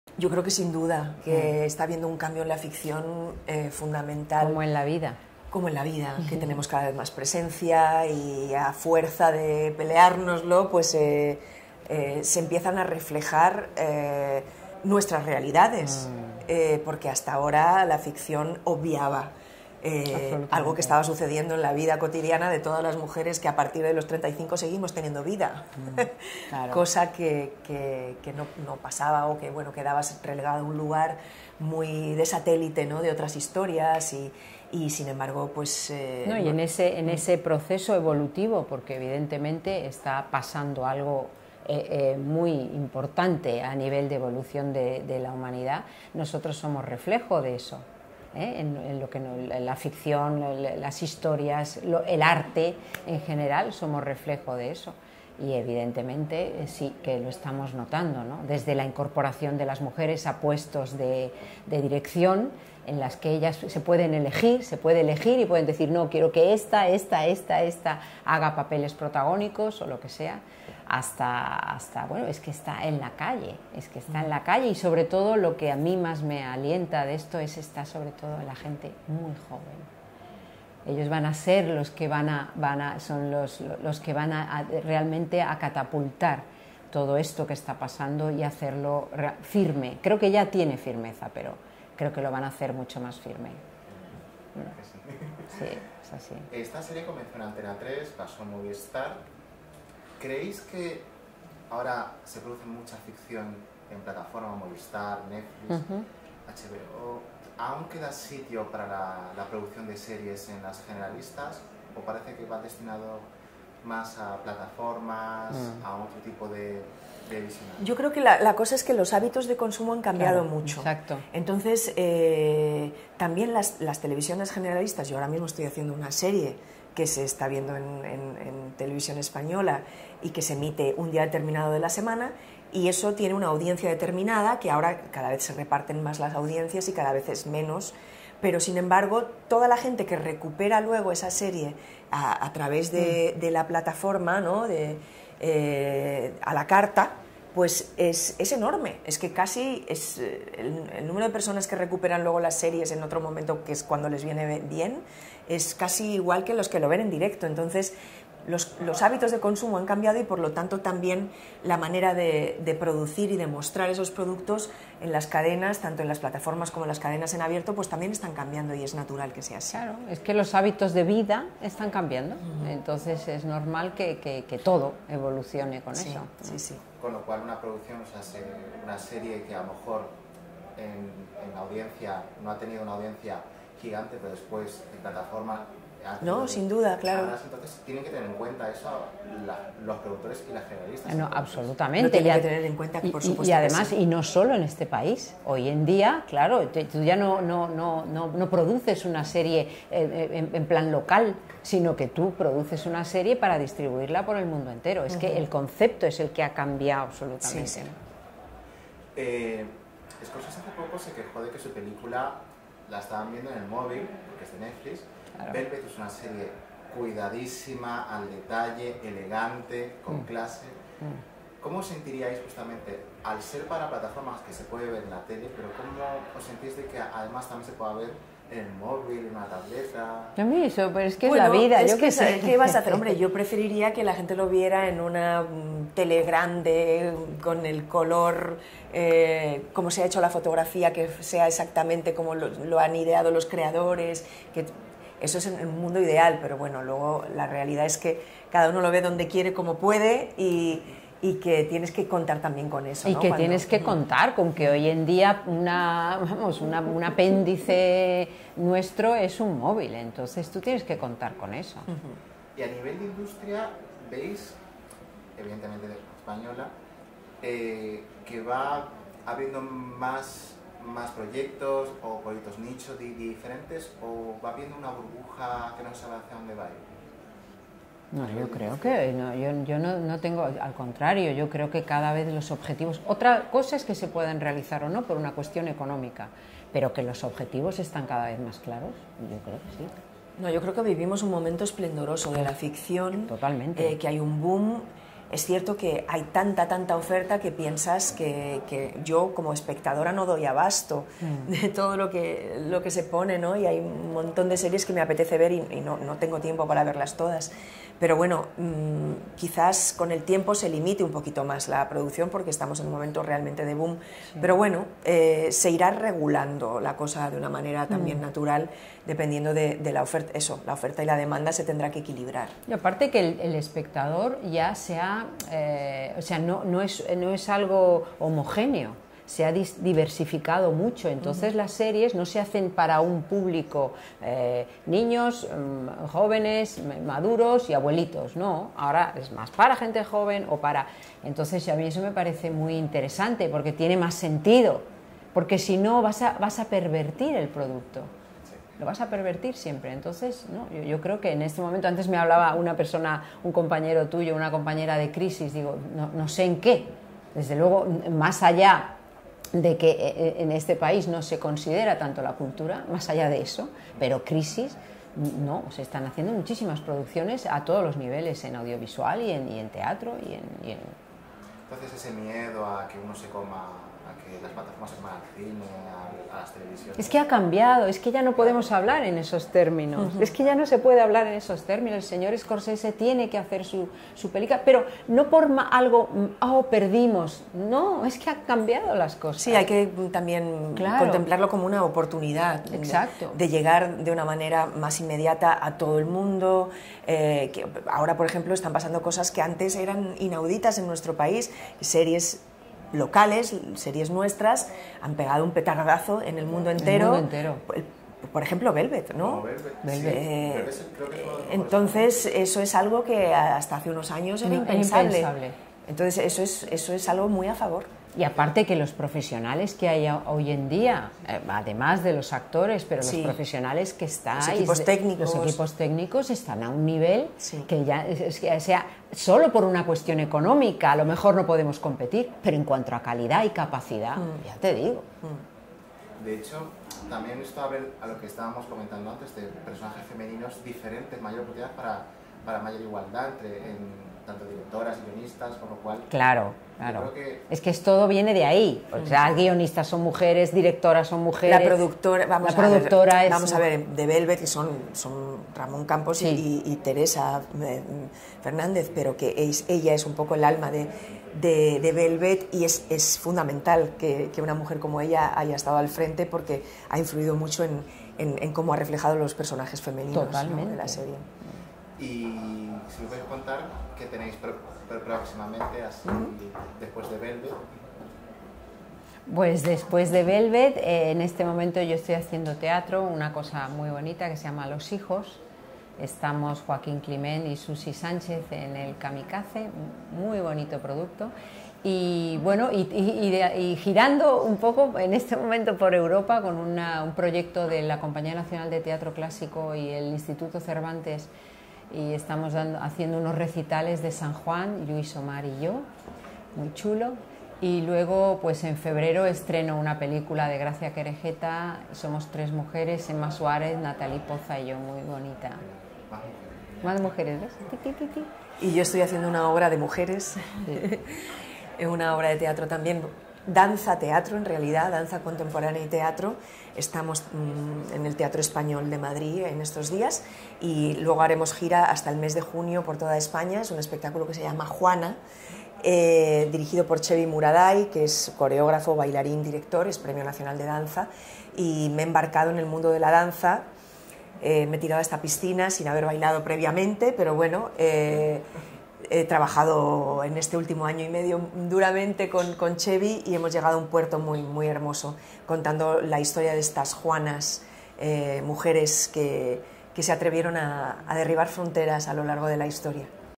Adriana Ozores y Aitana Sánchez-Gijón, sobre papeles femeninos en la ficción
Declaraciones de las actrices Aitana Sánchez-Gijón y Adriana Ozores. Aitana Sánchez-Gijón ha señalado ha señalado que "está habiendo un cambio en la ficción fundamental" en el tratamiento de los personajes femeninos y que las mujeres tienen "cada vez más presencia".